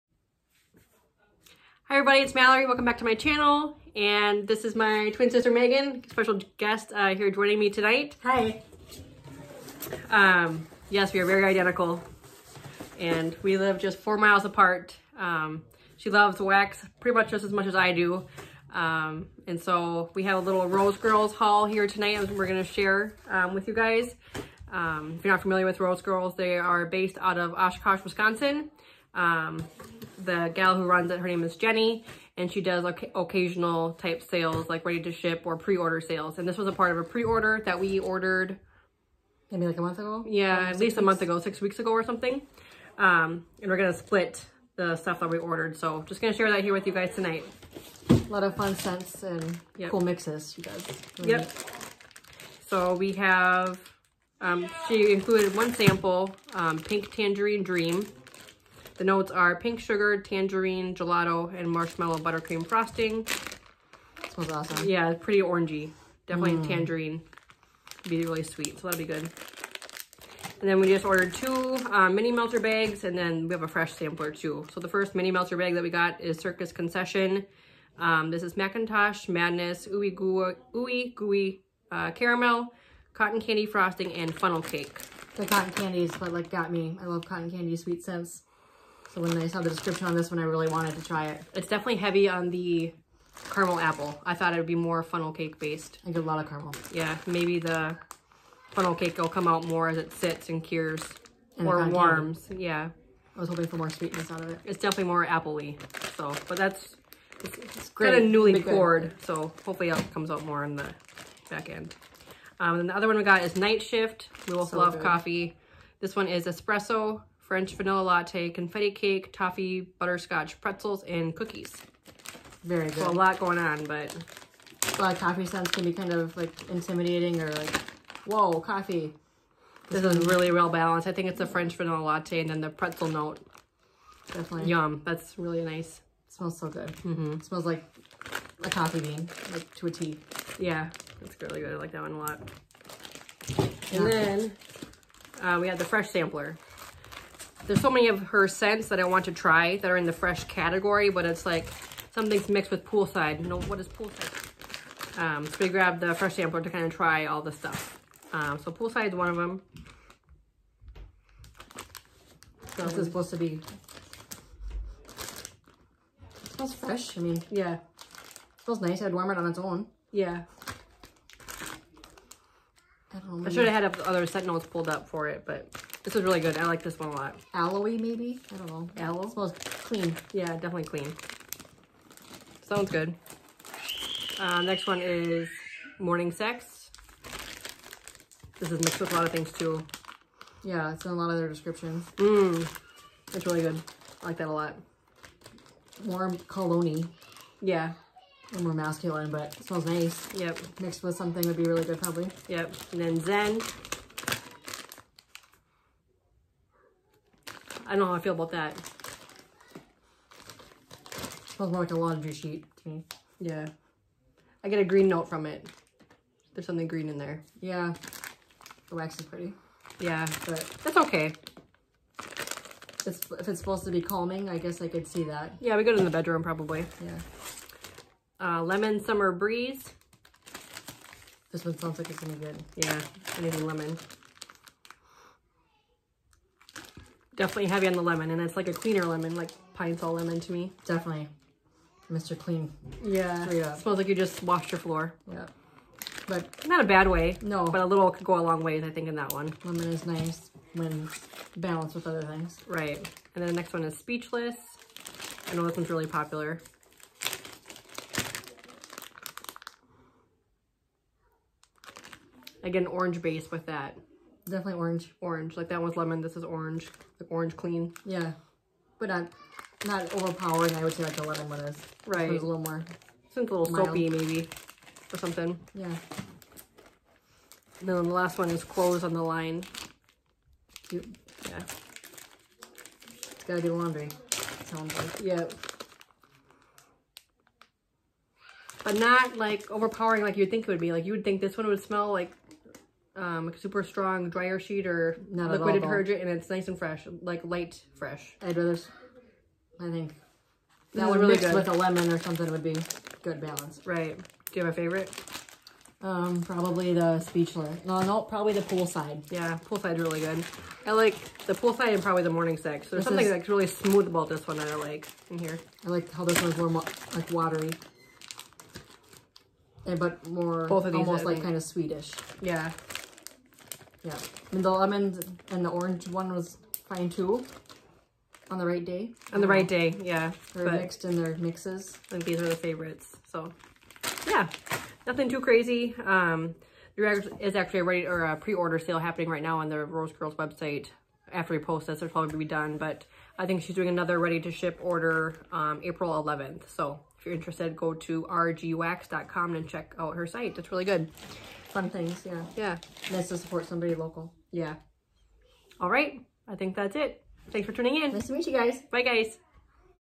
Rose Girls Feb 2022 Pre-order Haul w/special guest Megan!
Wanted to share with you our recent pre-order haul from Rose Girls Wax! I will link their website below!
*APOLOGIES for the brief interruption in the middle of the video as well as any background noise, we were hosting a birthday party. 🙂